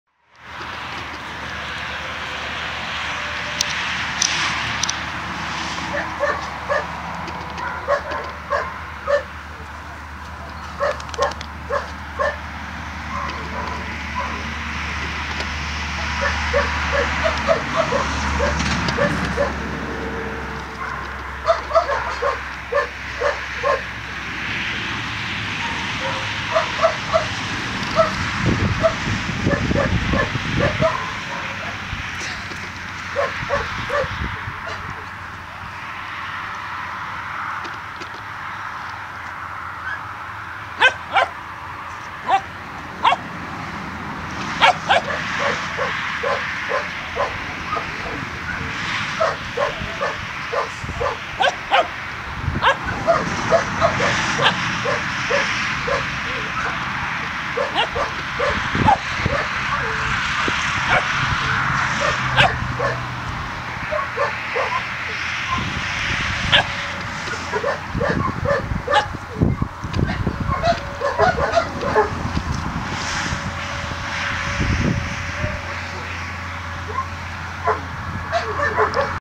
Working Kelpie puppy - Reef - playing
Our working kelpie puppy reef is playing with some members of her pack Nemesis (Gamma des crocs de l'olympe) Dutch Shepherd and Aura(Wibli aslihan) Dobermann at Drive Unlimited C.S.C. field.